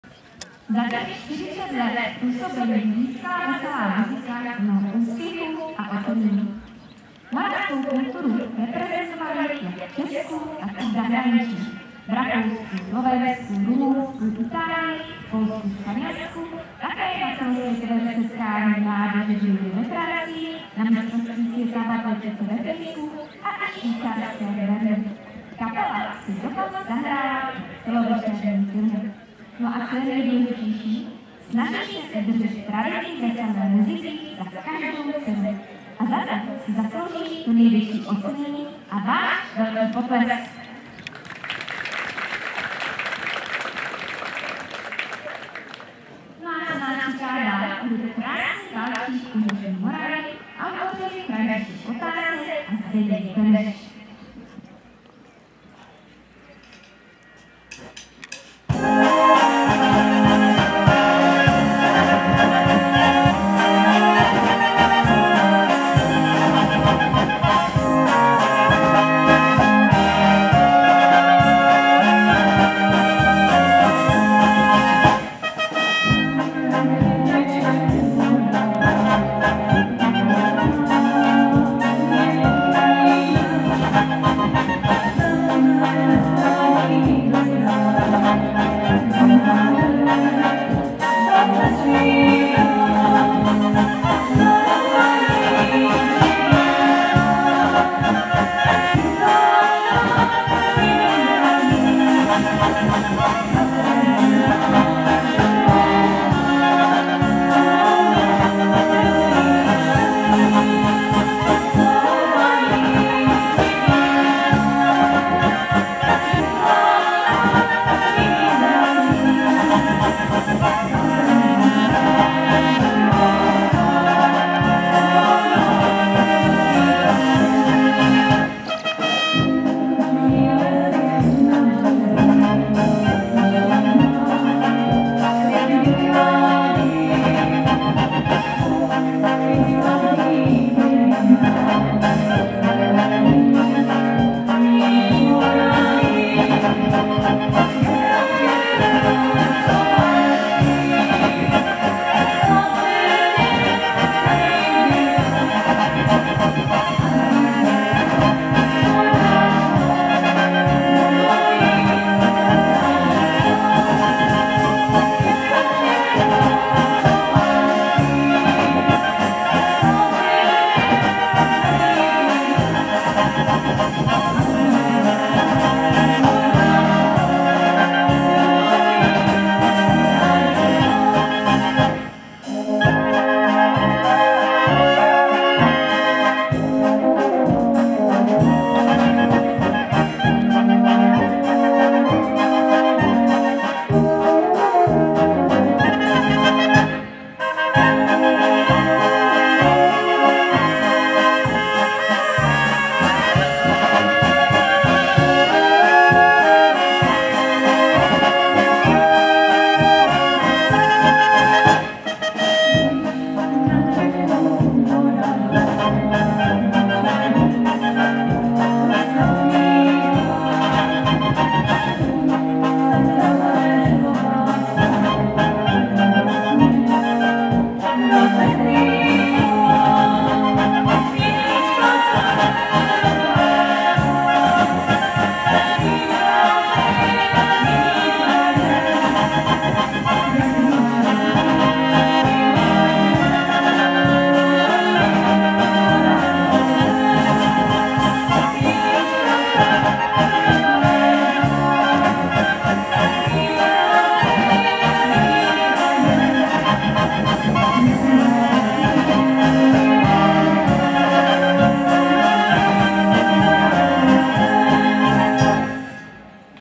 RATÍŠKOVICE-53.MEZINÁRODNÍ FESTIVAL DECHOVEK (Veselá muzika z Ratíškovic 2.)
- 11. července 2021, 11,00 hodin-18.30 hodin, fotbalový areál v Ratíškovicích (Kyjovsko)